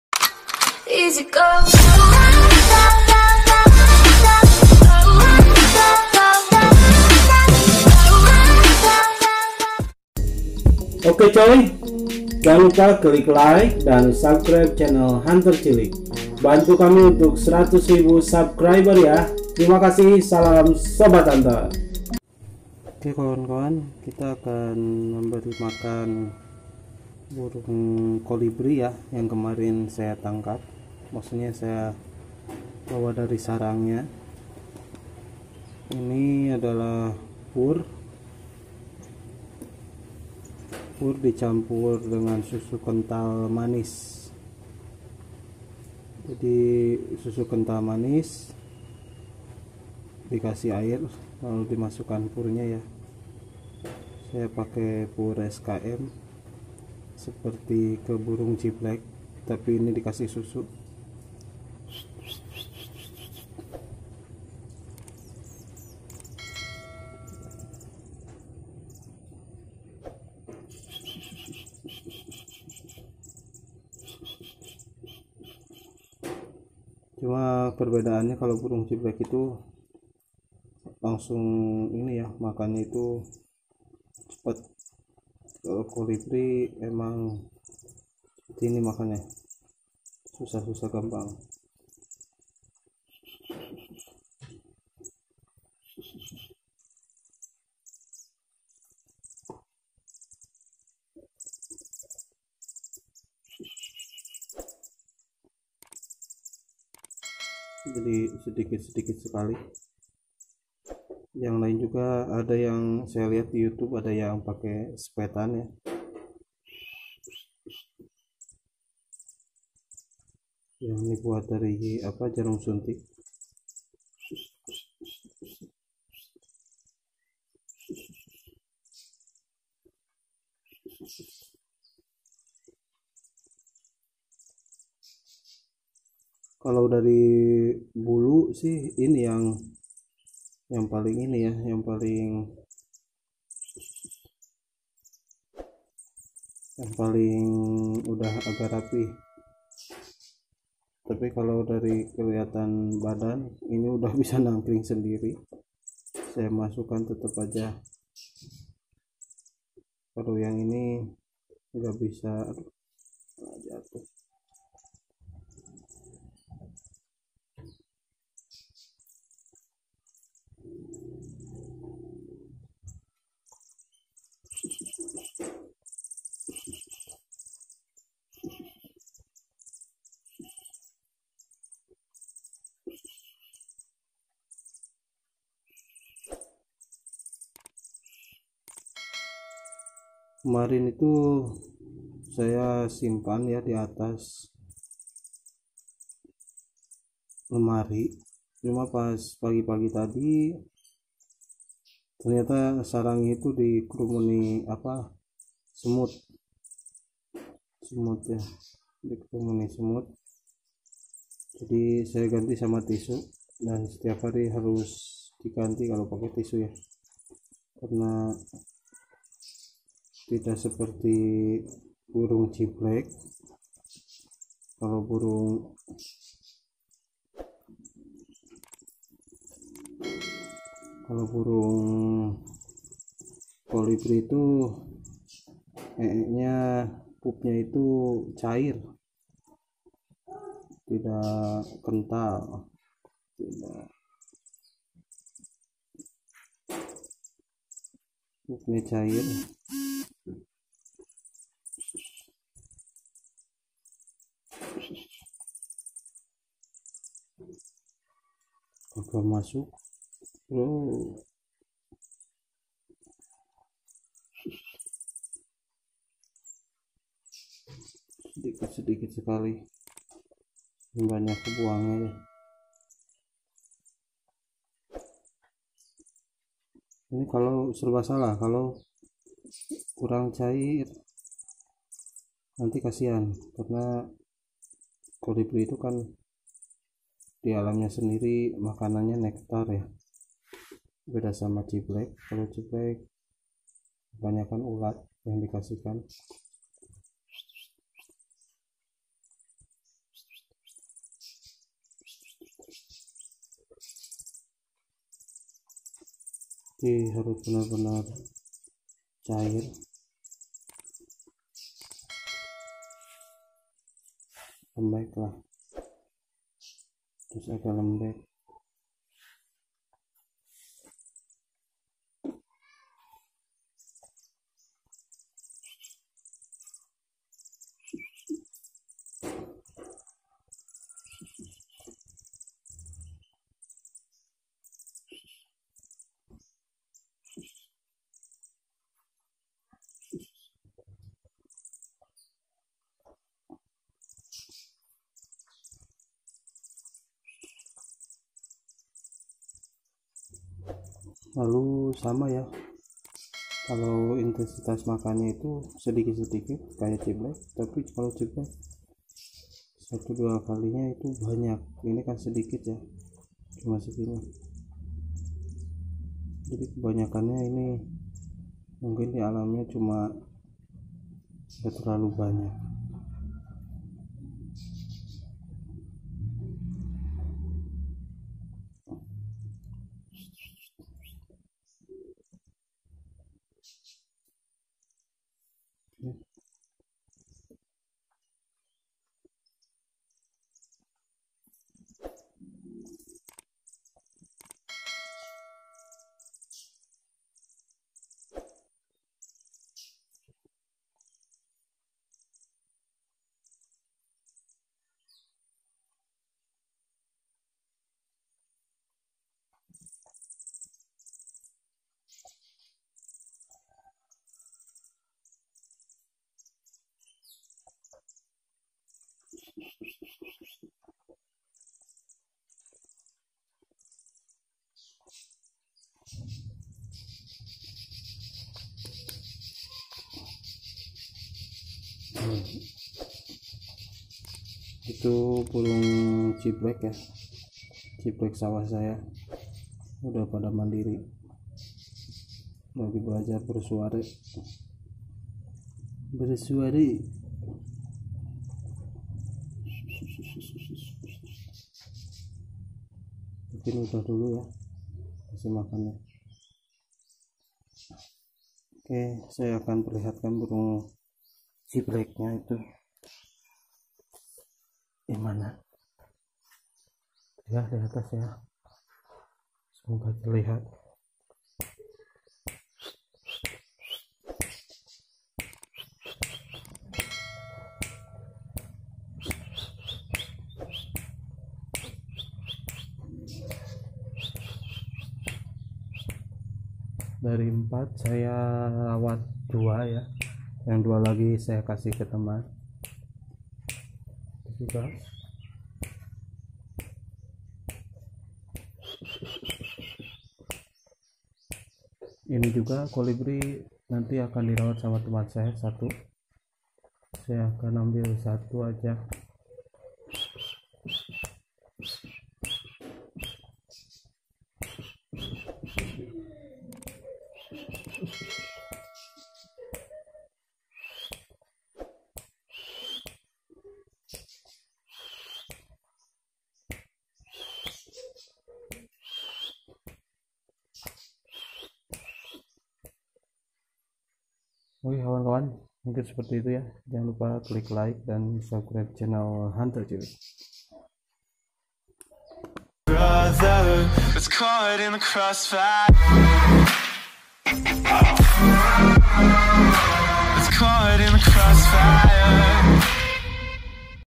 MERAWAT MELOLOH ANAK KOLIBRI
hunter cilik kali ini memposting video meloloh burung kolibri sebelum di ambil kawan saya lets play be happy yah cuyyy